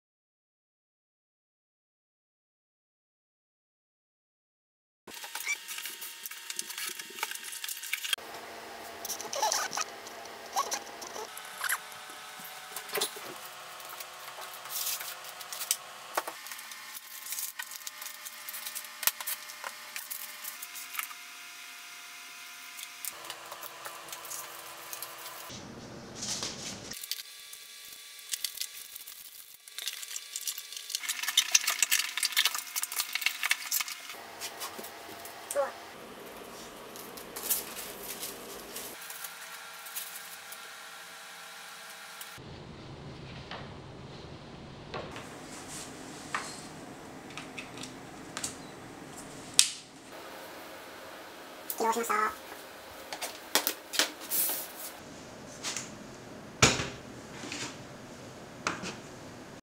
Mac整備済品 MacBookPro2018 1分開封
15.4インチMacBook Pro 2.9GHz 6コアIntel Core i9 RetinaディスプレイおよびRadeon Pro Vega 16搭載モデル - シルバー [整備済製品]

整備済製品買ったので開封動画作ってみました。